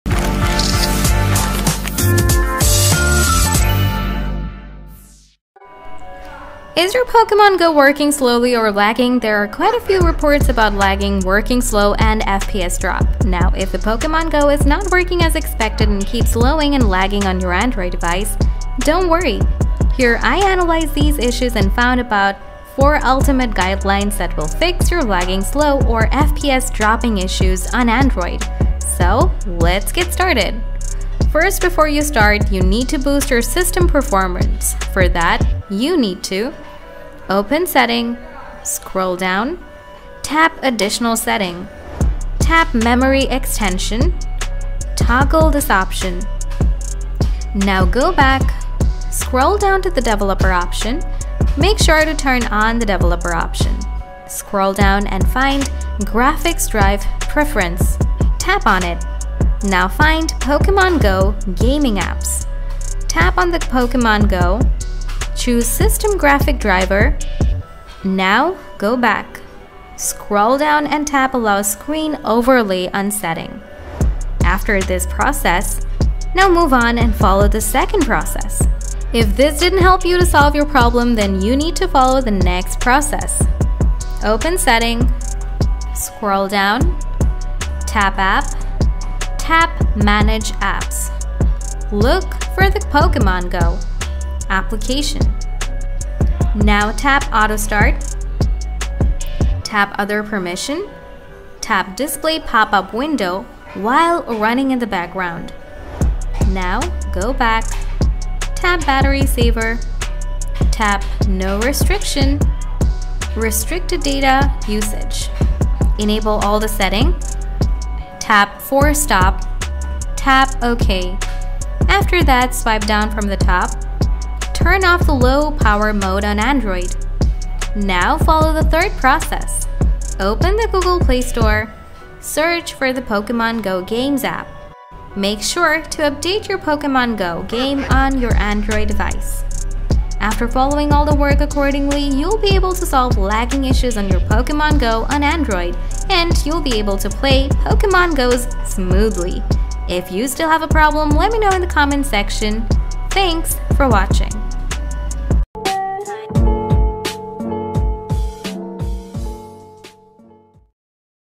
How To Fix Pokemon Go Lagging/Slowing Issue on Android || Play Pokemon Go without Lag
Is your Pokemon GO working slowly or lagging? To fix pokemon go lag or make pokemon go run smoother on android, keep watching this video.
Now, if you found that the Pokemon lagging or running slow on Android Then to fix pokemon go lag or hang problem then don't worry! Let's go ahead and see how to fix pokemon go lagging and running slow issue on Android.
So after analyzing the matter, here i am going to present some effective solutions that will help you to Play Pokemon go without Lag and also fix lagging or slowing issues on Android!

00:01- Video intro
00:35- Solution 1: Boost your system performance
01:24- Solution 2: Remove software glitches from the app
02:05- Solution 3: Turn off low-power mode
02:12- Solution 4: Update Pokemon go from the play store
02:34- Video ending